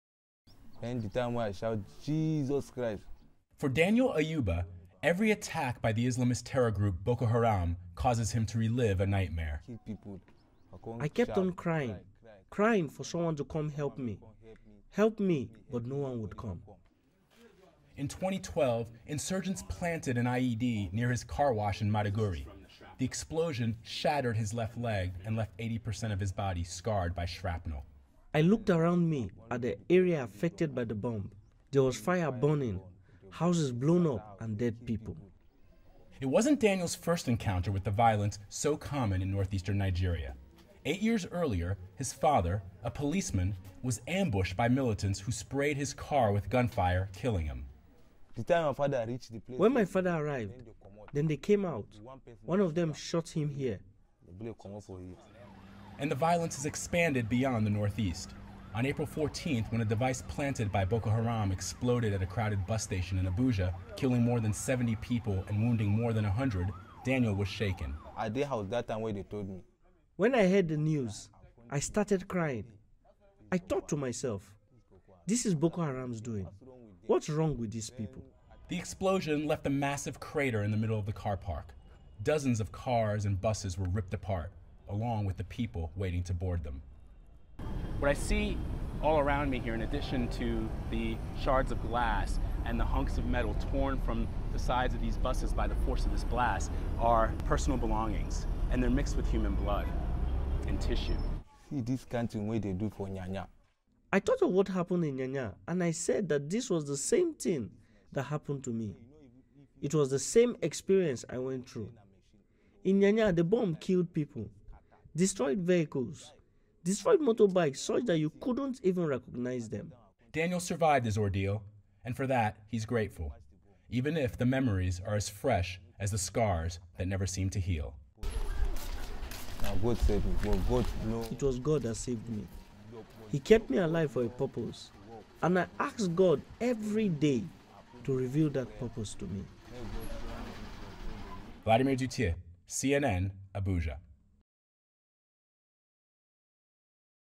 NIGERIA SURVIVOR'S TALE OF BOKO HARAM VIOLENCE
Survivor's tale of attack by insurgents one of many in long trail of Nigerian violence. CNN's Vladimir Duthiers reports.